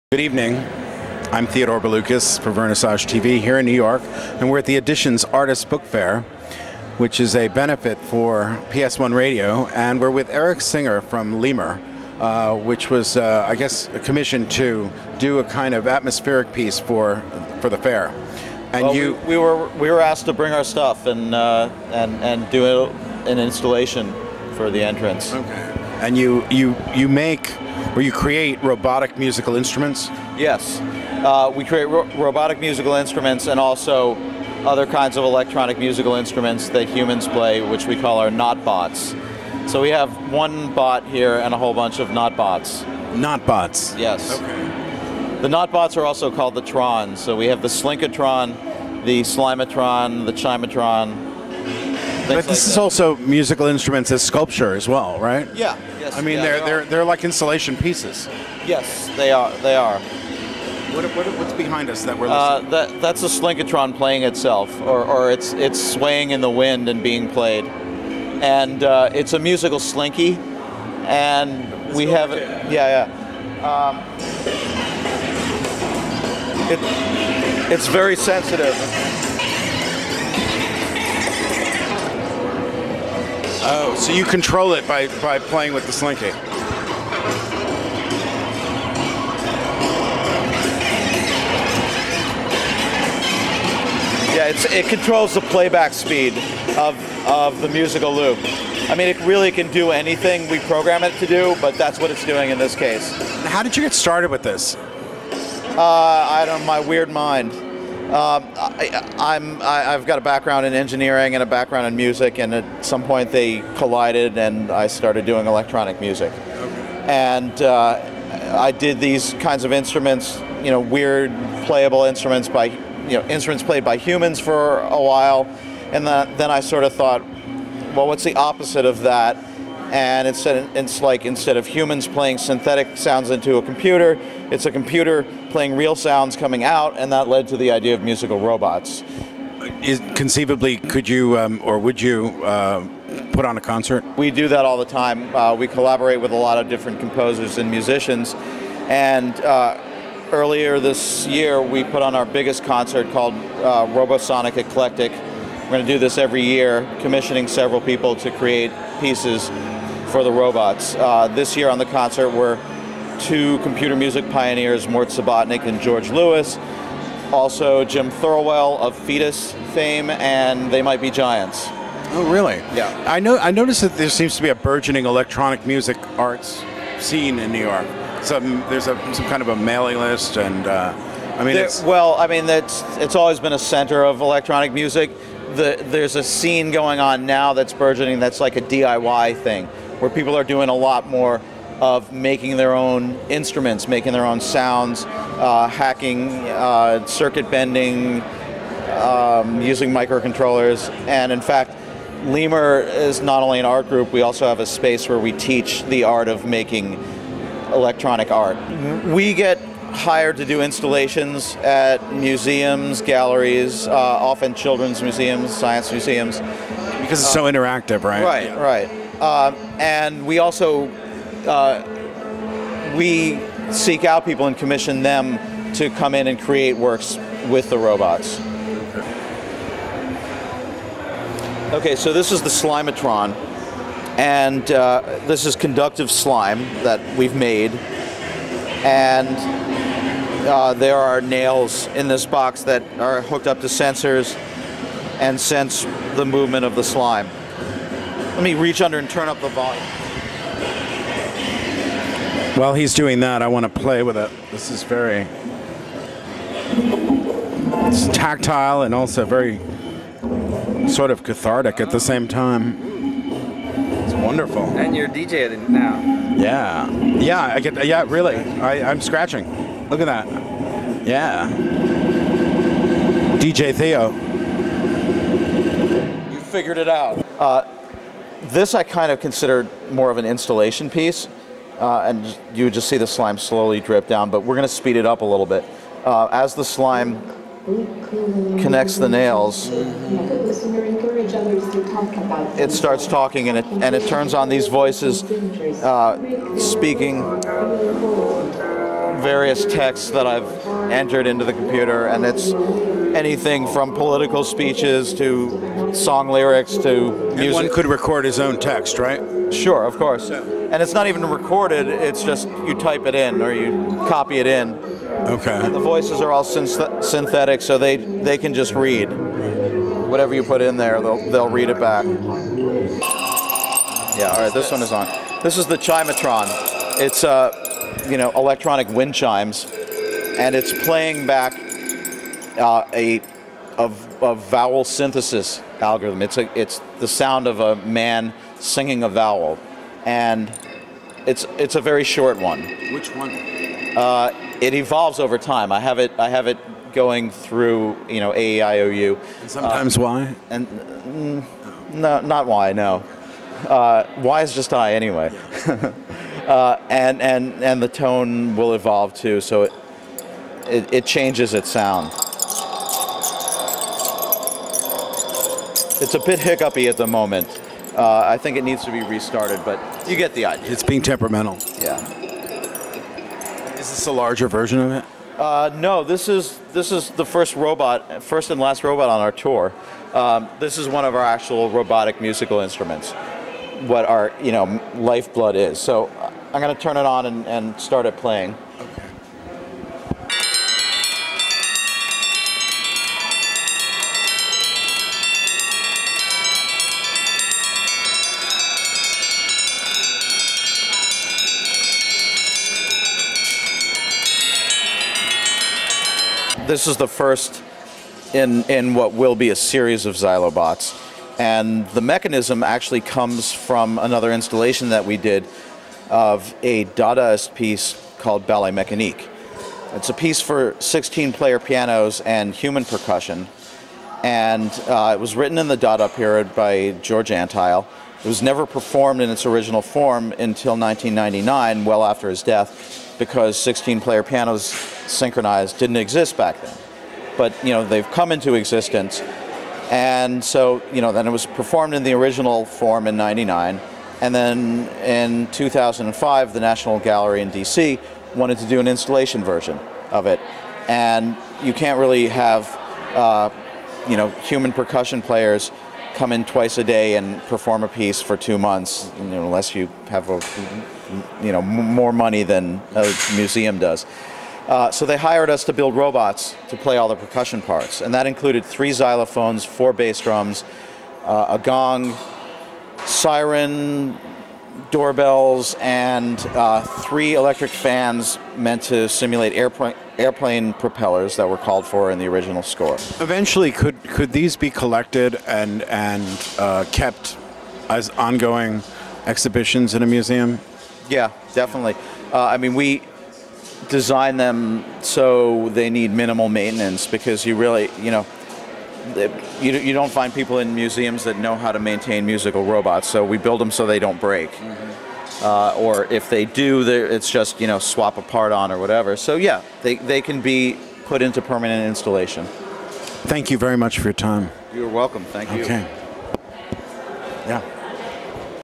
Eric Singer, founder of LEMUR at the Editions/Artists' Book Fair
Eric Singer’s LEMUR – the League of Electronic Musical Urban Robots – is a Brooklyn-based group of artists and technologists who create robotic musical instruments. Founded in 2002, LEMUR recently performed at the Opening Night Gala of the Editions/Artists’ Book Fair (E/AB), benefiting Art Radio WPS1.org, the Web radio station of P.S.1 Contemporary Art Center. VTV’s New York correspondent Theodore Bouloukos met up with Eric Singer, a musician and engineer, and LEMUR’s founder, who gives us an insight into the world of robotic musical instruments. LEMUR builds robots as new types of musical instruments, as opposed to animatronic robots that play existing instruments. Now in its 10th year, the E/AB Fair is the premier showcase for contemporary publishers and dealers presenting the latest and greatest in prints, multiples, and artists’ books. The Editions/Artists’ Book Fair Opening Night Gala, New York, November 1, 2007. Camera: Darren Trenchard.